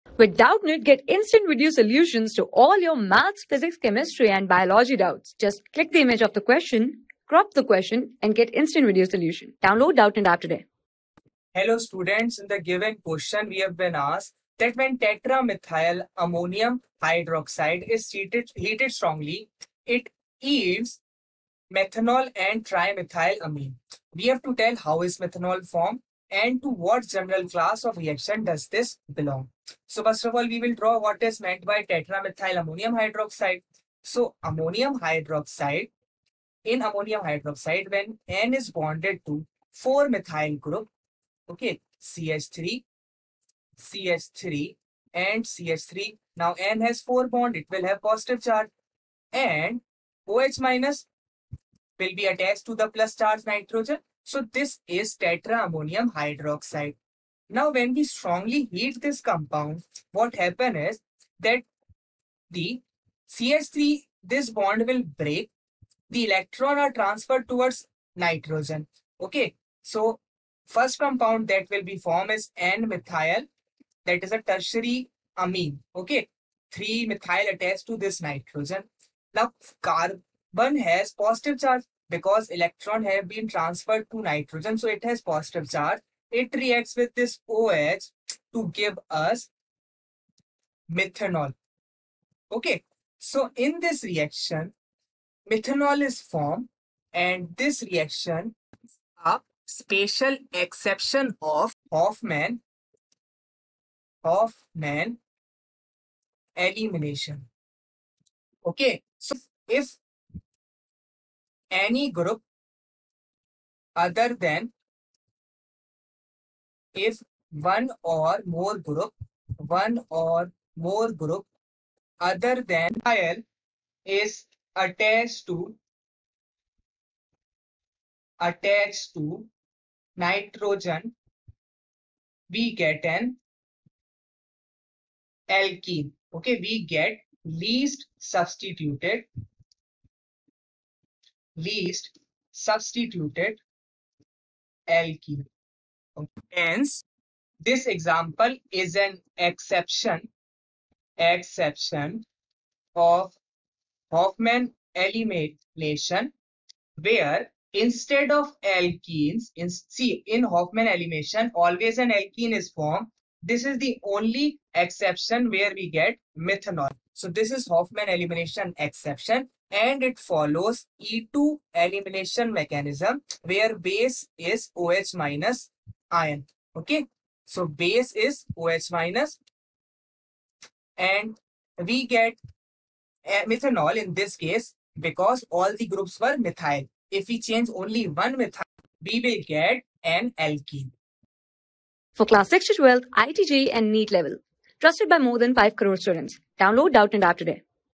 When tetramethyl ammonium hydroxide is heated strongly, it yields methanol and trimethylamine . ...
When tetramethyl ammonium hydroxide is heated strongly, it yields methanol and trimethylamine . How is methanol formed : To what general class of reaction does this belong ?
Class: 12
Subject: CHEMISTRY
Chapter: ORGANIC COMPOUNDS WITH FUNCTIONAL GROUP
Board:IIT JEE

👉 Have Any Query? Ask Us.